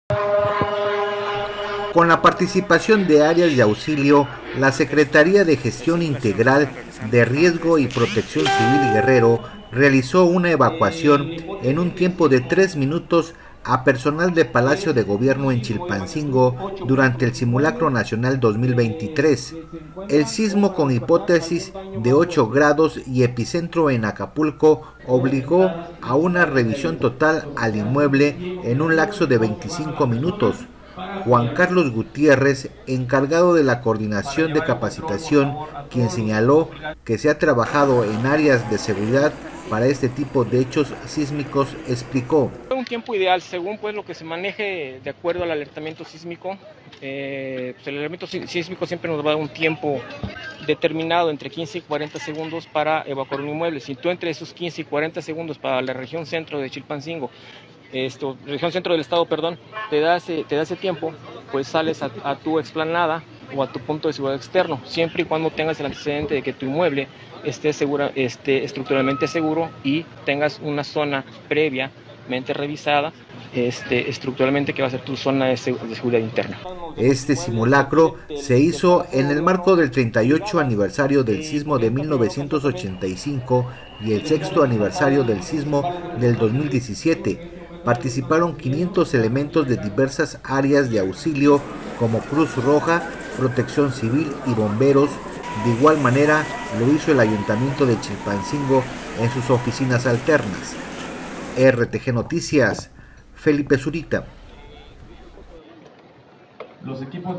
RTGnoticias | Evacuan en 3 minutos Palacio de gobierno durante el simulacro Nacional 2023
Con la participación de 500 áreas de auxilio, la Secretaría de Gestión Integral de Riesgo y Protección Civil Guerrero, realizó una evacuación en un tiempo de 3 minutos a personal de Palacio de Gobierno en Chilpancingo, durante el Simulacro Nacional 2023.